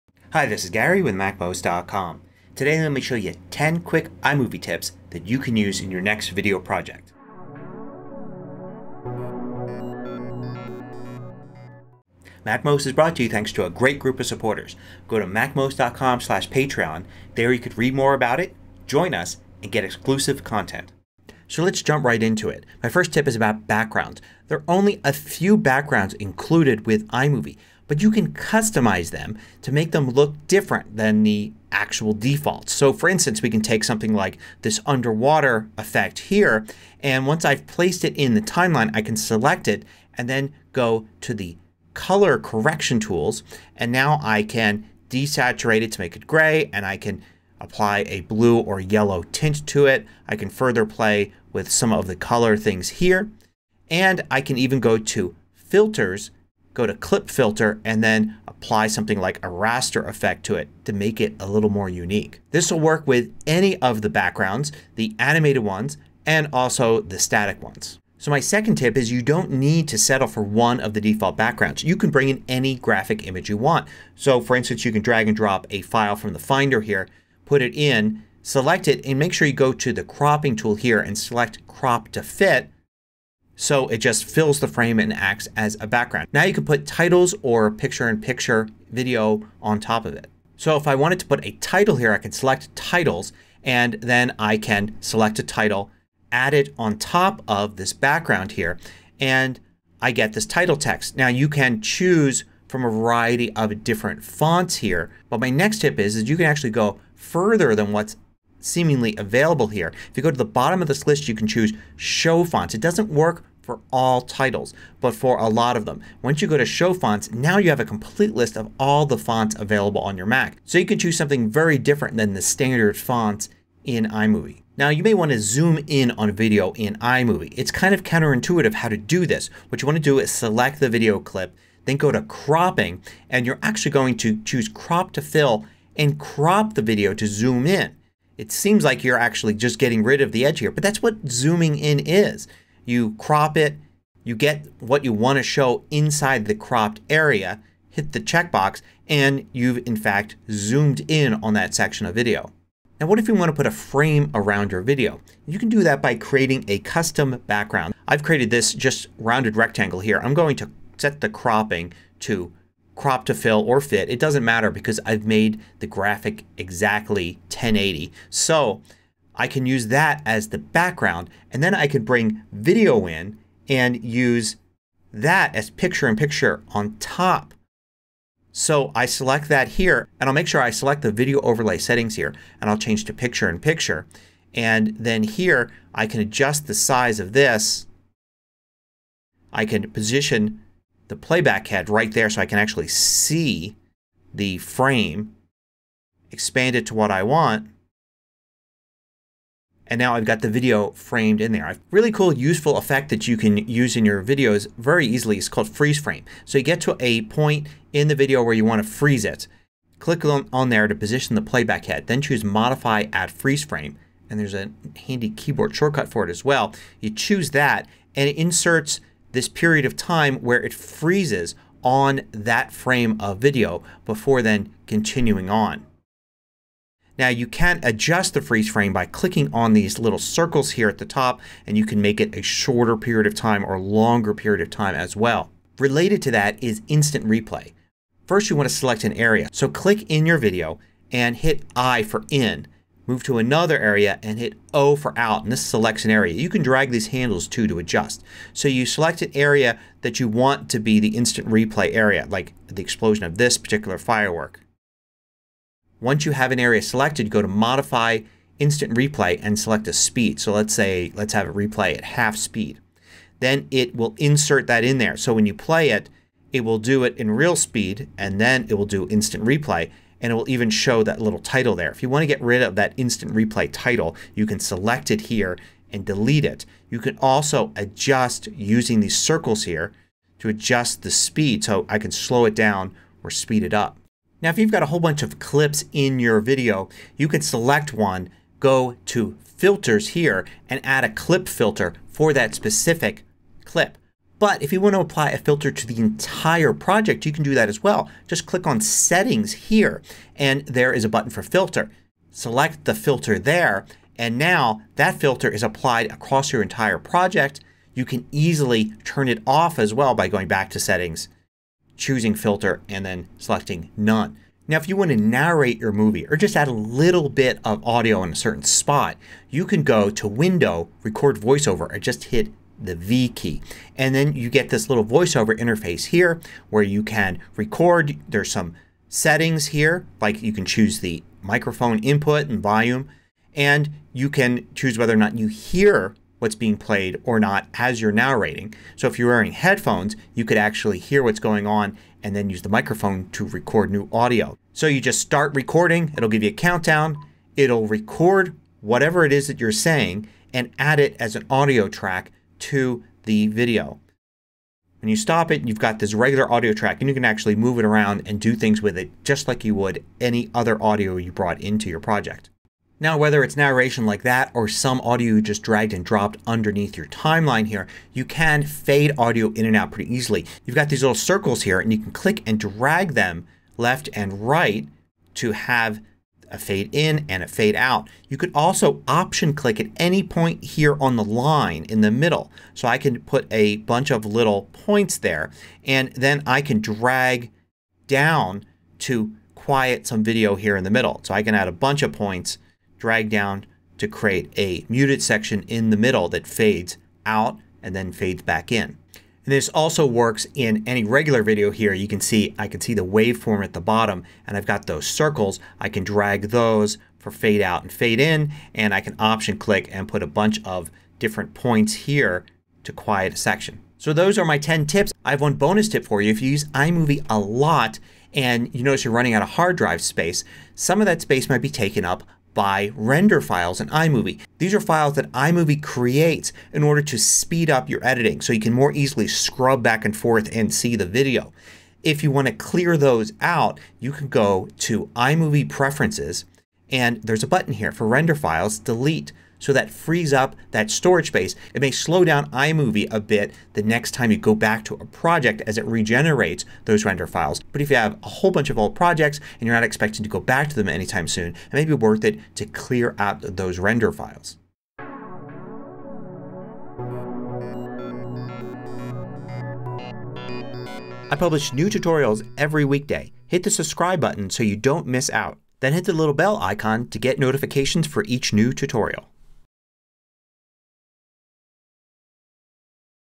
10 iMovie Tricks To Use In Your Next Video Project
Learn some quick and simple tricks you can use to enhance your next iMovie video project. You can adjust iMovie's backgrounds to make them unique or bring in your own. Add audio narration and adjust the fade in and out of any audio. Use cropping to zoom in and special effects like freeze frame and instant replay.